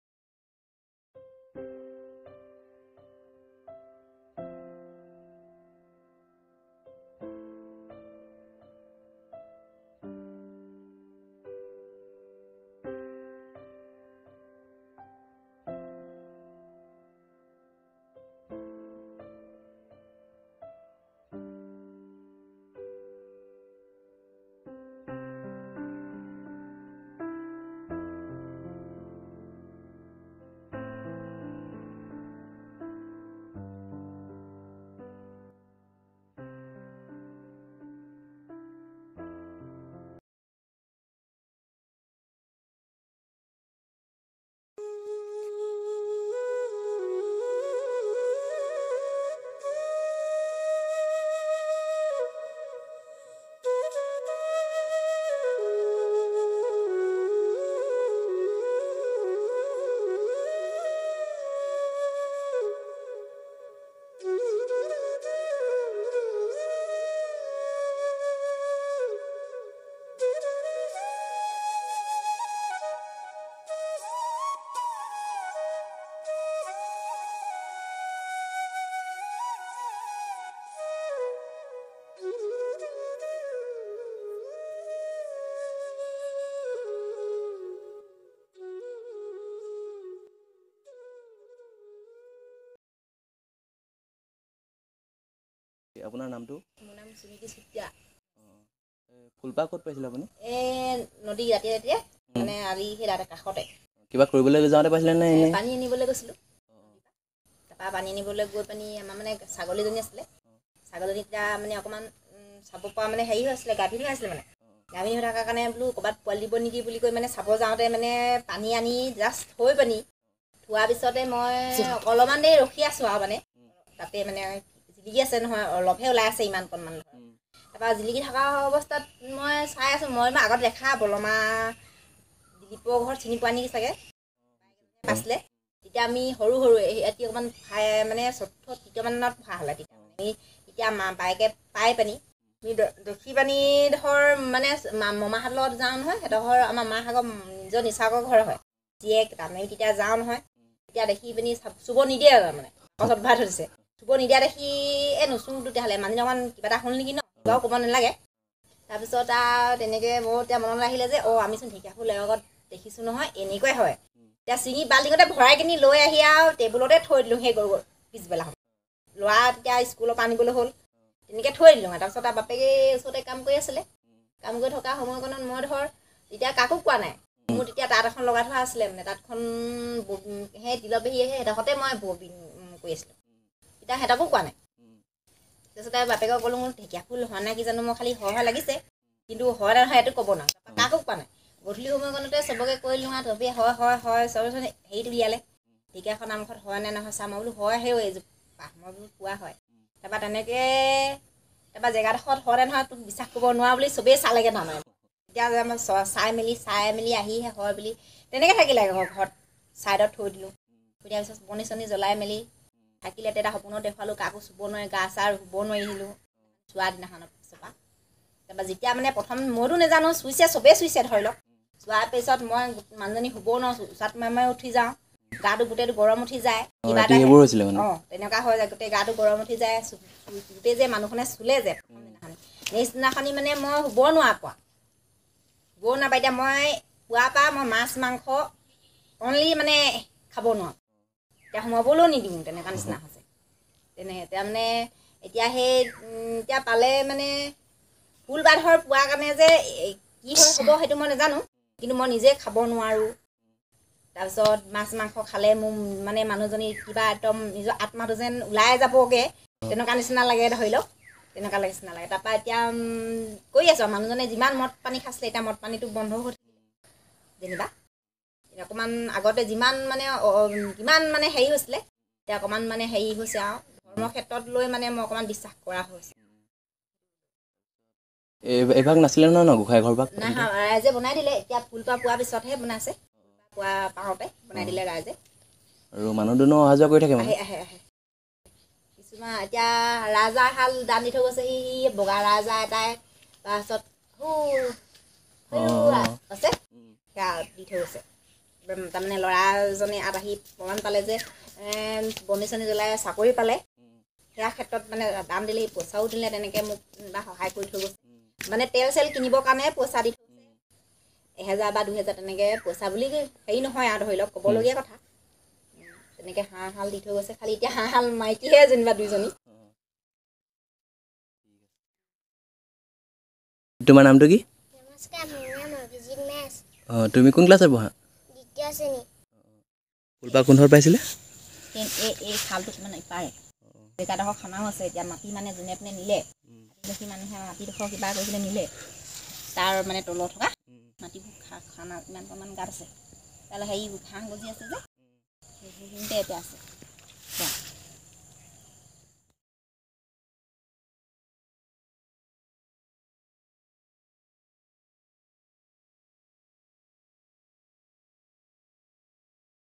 FIDDLEHEAD FERNS FLOWER || DOCUMENTARY || BISHAL DUARAH
Documented a superstition performed in Assam.
Have a look and share your feedbacks.